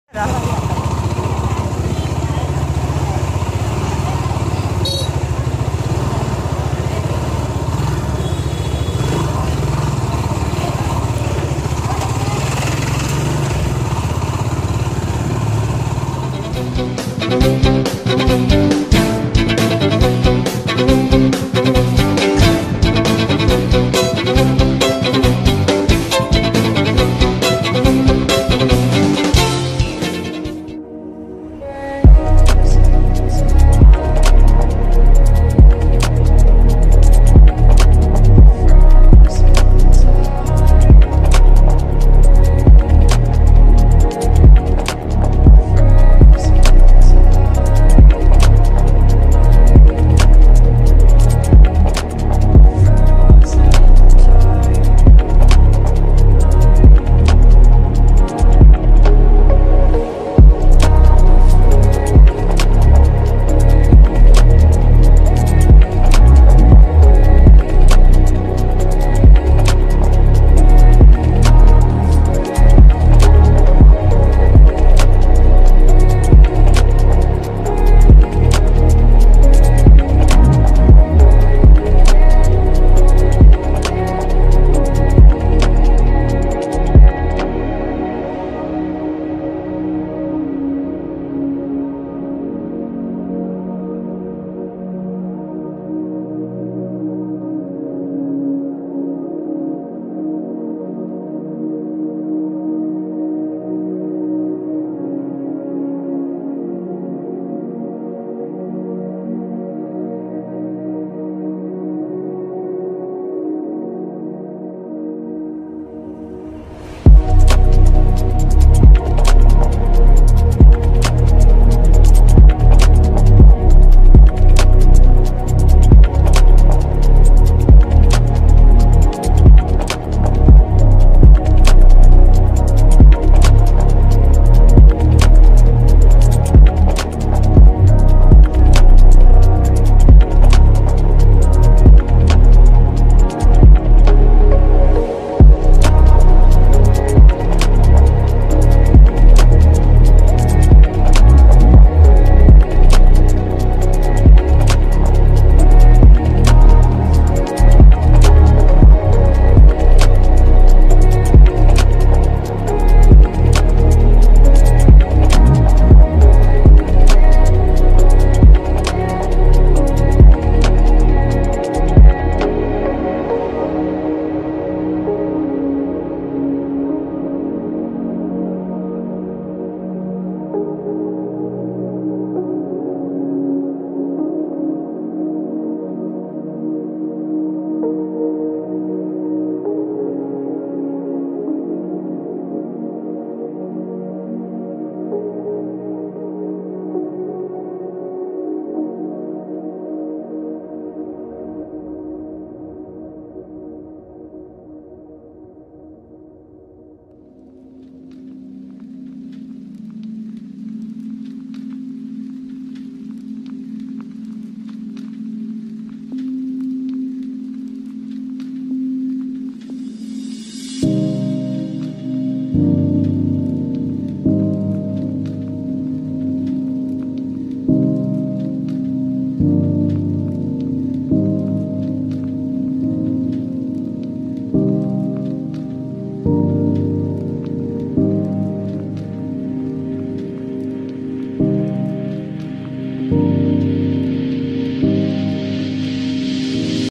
[ S1 - EPS. 2 ] NOWHERE!!
Currently we have just Entered into a Village,  and people in Village staring us like us like , they have seen a Alien , 😂😂... I love the Reactions of people around me.